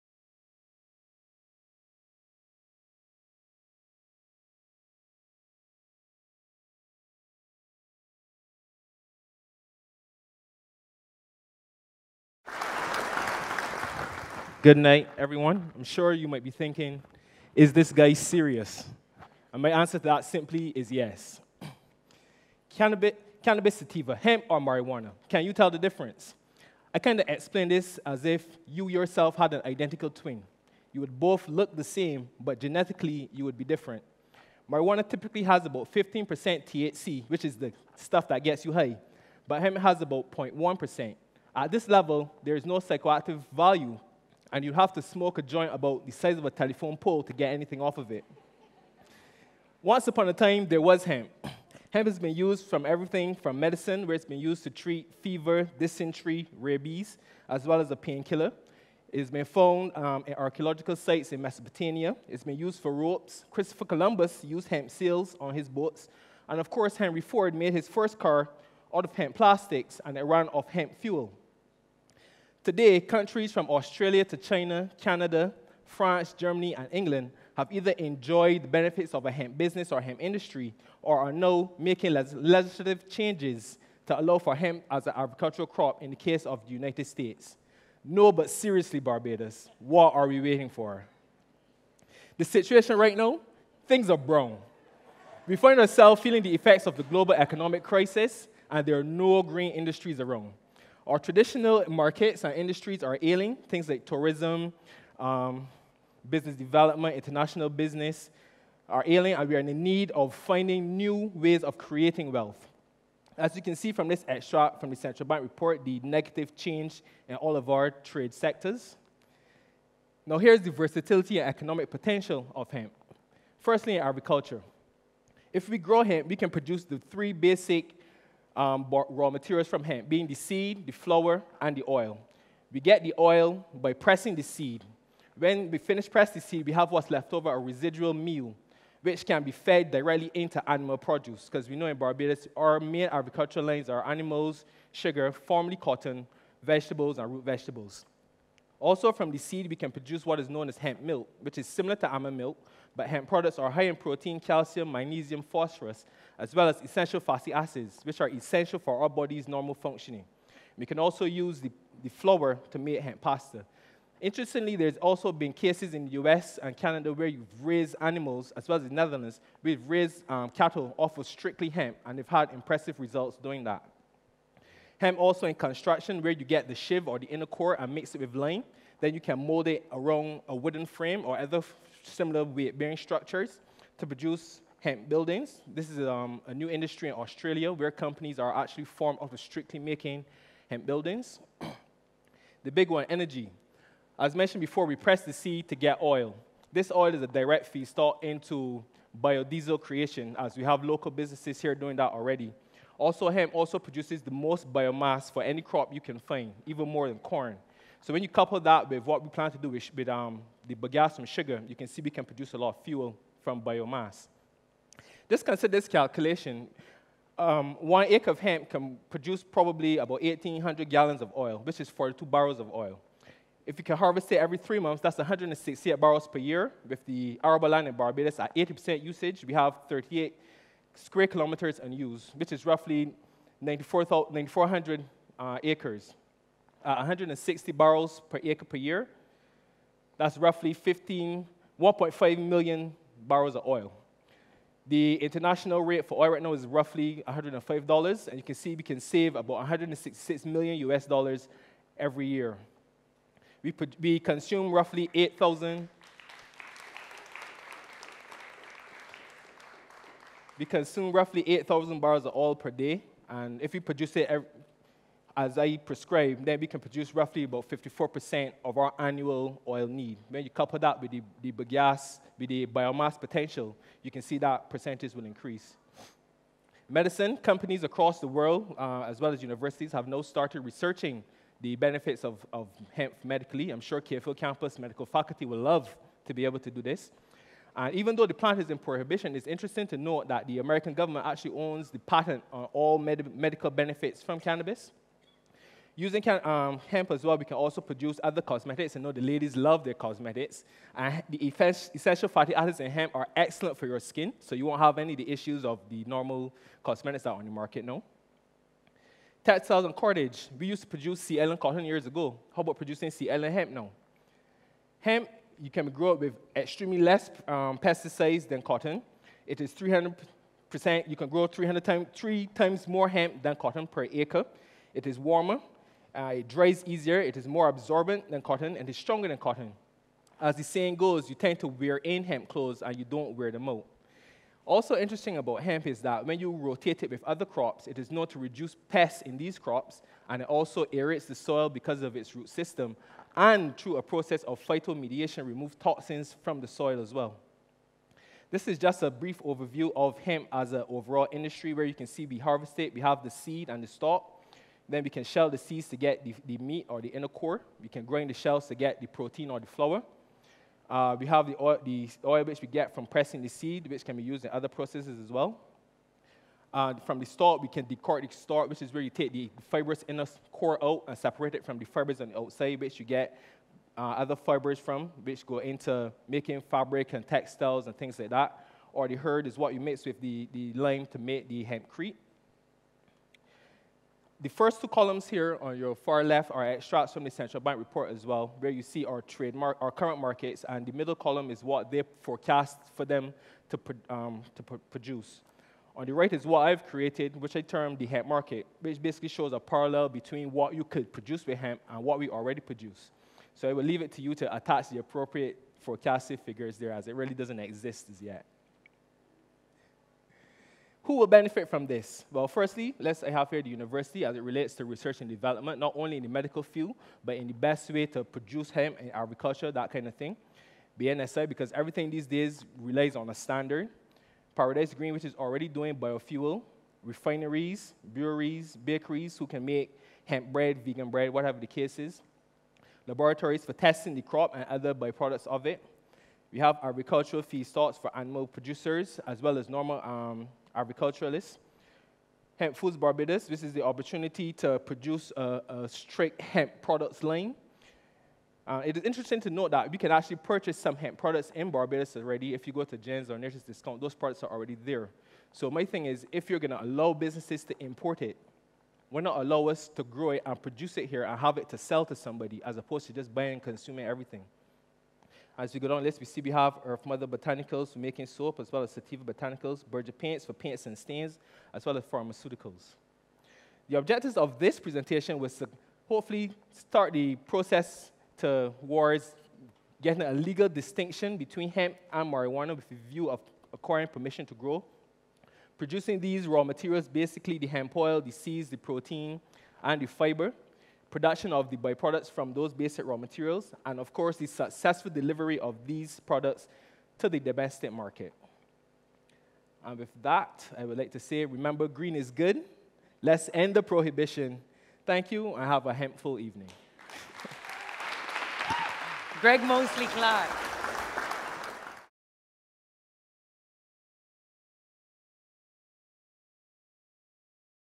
Hemp the trillion dollar crop: Gregg Moseley-Clarke at TEDxBridgetown
Gregg Moseley-Clarke is a graduate of the University of the West Indies and the University of Hull (UK). He holds a Bachelors of Science degree in Computer Science and Accounting and a MBA in Information Management. He currently holds the position of Human Resources Analyst at the University of the West Indies Cave Hill Campus. With over 10 years experience in Insurance and banking industries, Gregg turned his focus to creating new and innovative industries, founding Hemp Foods Barbados and Sateva Botanicals, 2 local companies focused on prompting hemp food products and locally made hemp cosmetics.He is also a former national rugby player.